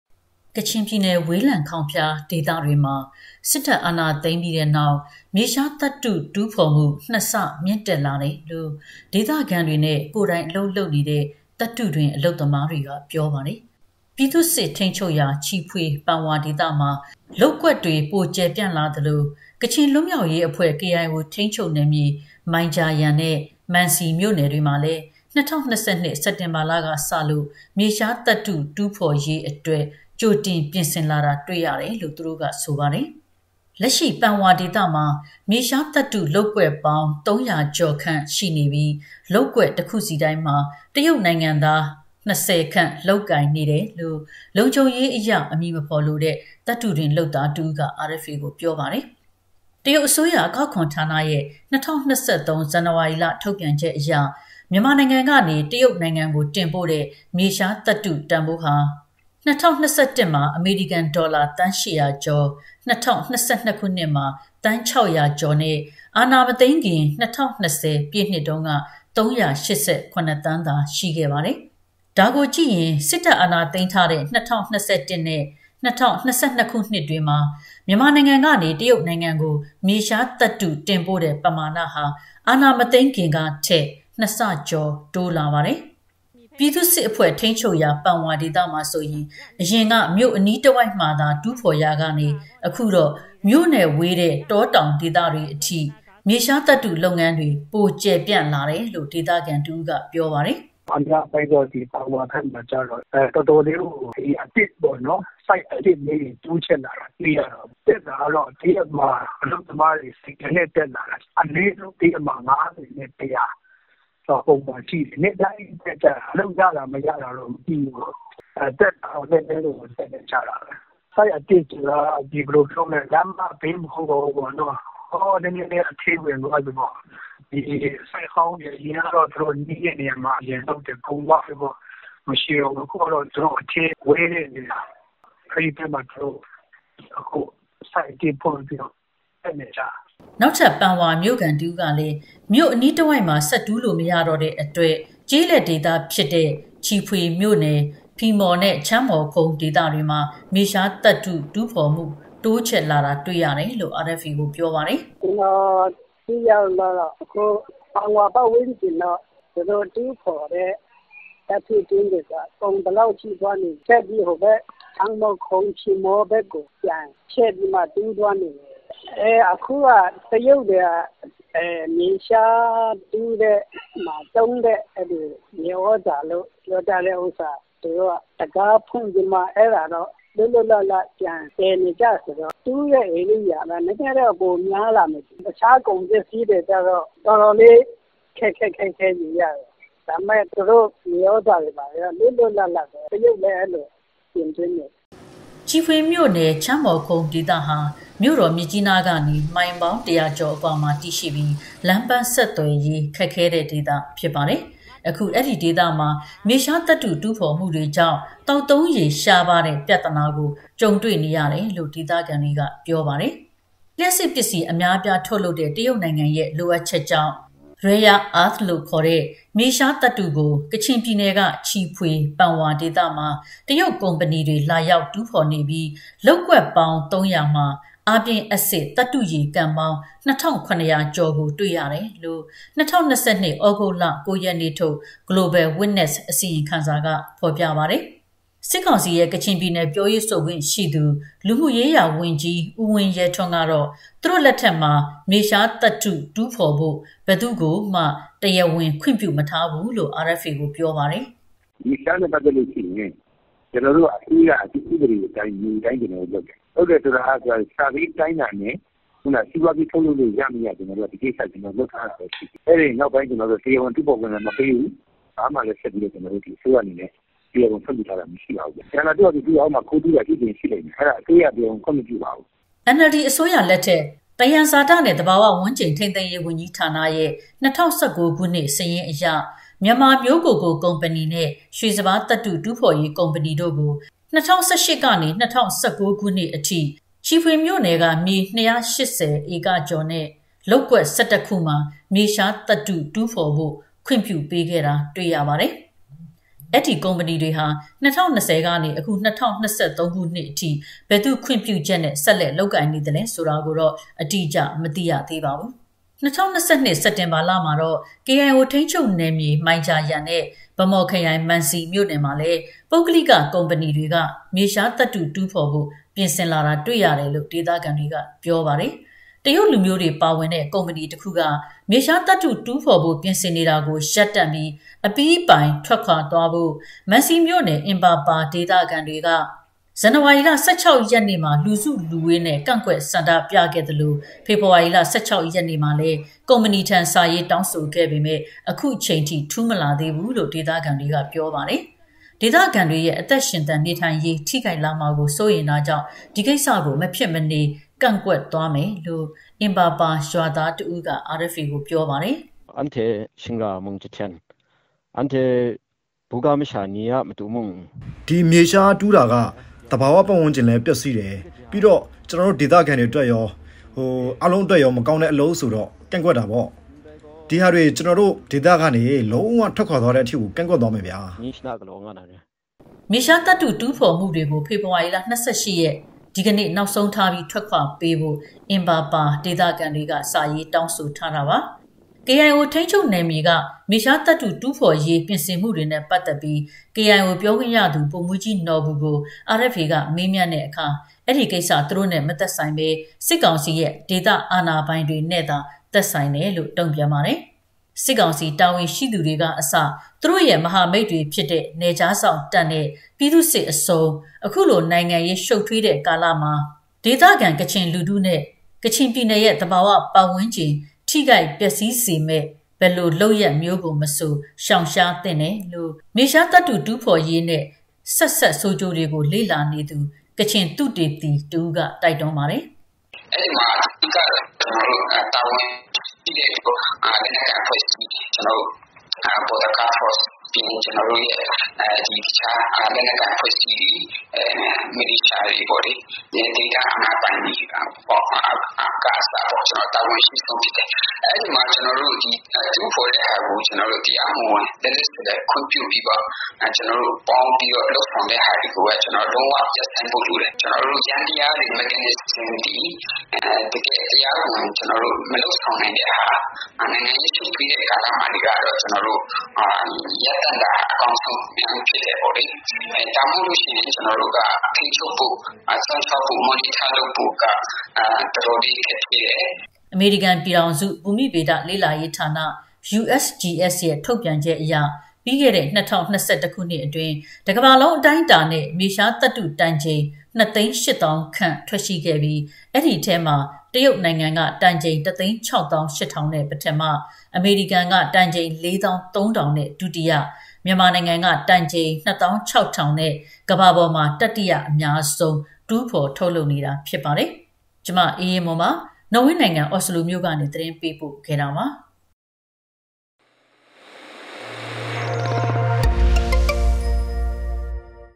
ကချင်ပြည်နယ်မှာ မြေရှားသတ္တုတူးဖော်မှု နှစ်ဆတိုးလာ
■ ကချင်ပြည်နယ် ဝေးလံခေါင်ဖျားဒေသတွေမှာ စစ်တပ်အာဏာသိမ်းပြီးနောက်  မြေရှားသတ္တုတူးဖော်မှု နှစ်ဆ မြင့်တက်လာတယ်လို့ ဒေသခံတွေနဲ့ သတ္တုတွင်း အလုပ်သမားတွေက ပြောပါတယ်။ အားပြင်းအက်စစ်တွေကို အသုံးပြုရတဲ့ မြေရှားသတ္တုတူးဖော်မှုဟာ ပြည်သူ့စစ်ထိန်းချုပ်ရာ ချီဖွေ၊ ပန်ဝါ ဒေသမှာရော ကေအိုင်အိုထိန်းချုပ်ရာ မန်စီမြို့နယ်မှာပါ ပိုများလာတဲ့အတွက် သဘာဝပတ်ဝန်းကျင် ထိခိုက်ပျက်စီးမှာကို စိုးရိမ်ပြီး ဒေသခံတွေကလည်း ဆန့်ကျင်ကန့်ကွက် နေပါတယ်။ သတင်းအပြည့်အစုံကို မအေးအေးမွန်က တင်ပြထားပါတယ်။